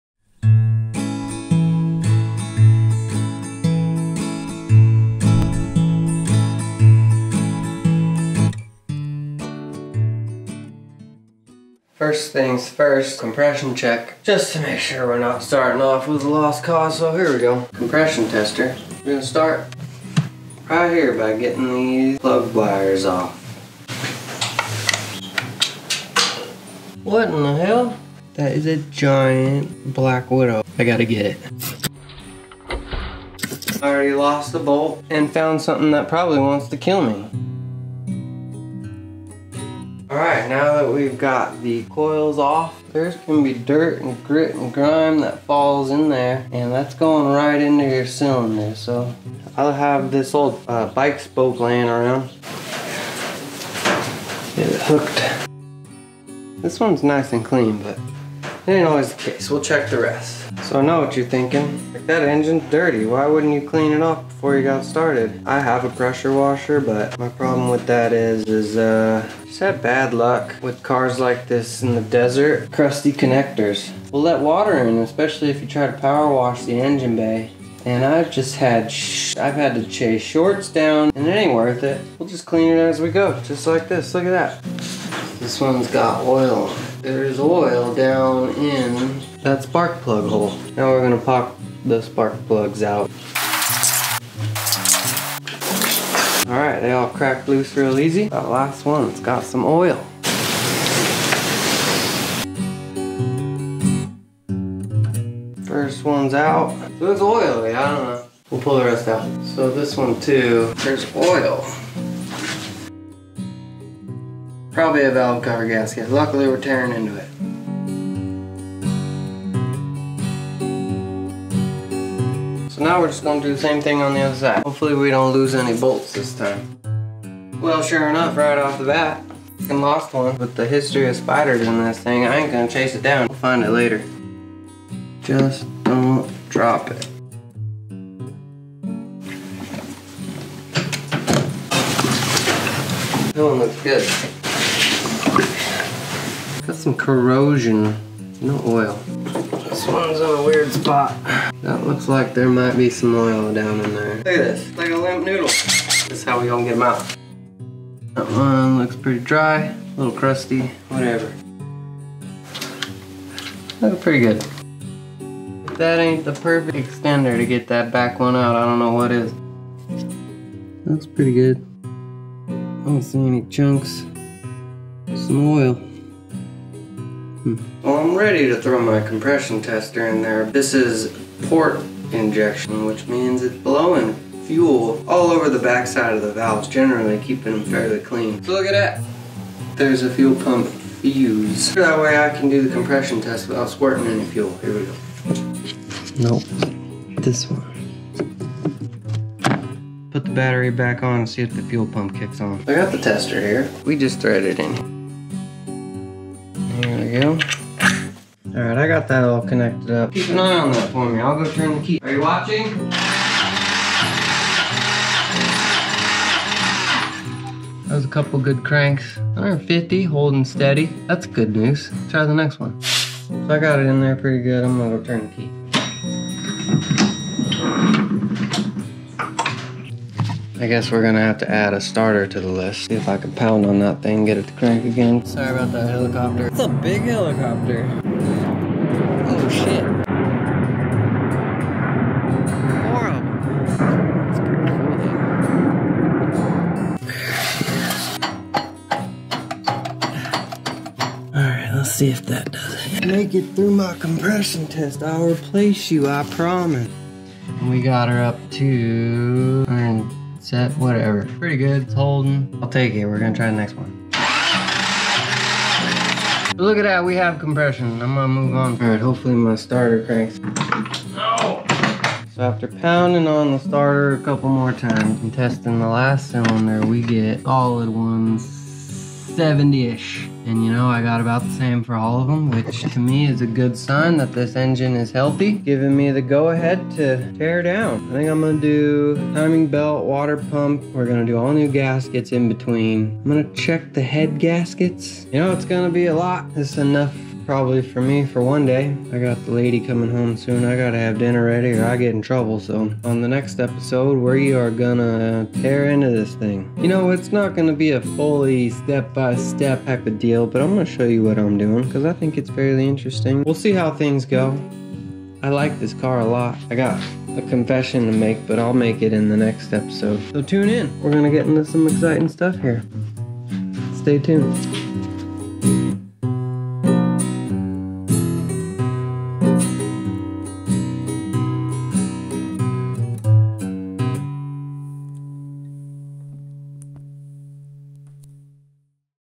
2000 Honda Passport Tune Up - Episode 2 Compression Test
In the last episode we did a shakedown of the 2000 Honda Passport 4x4 5 Speed to determine what needed to be addressed in order to make it a daily fun driver.

In this episode, we get it into the barn and test the compression to make sure we are not starting with a lost cause.

Tune in for episode 3 where we get a quick review on my Old Honda Passport that we will be using for parts.

Music is recorded by me hope you like it!!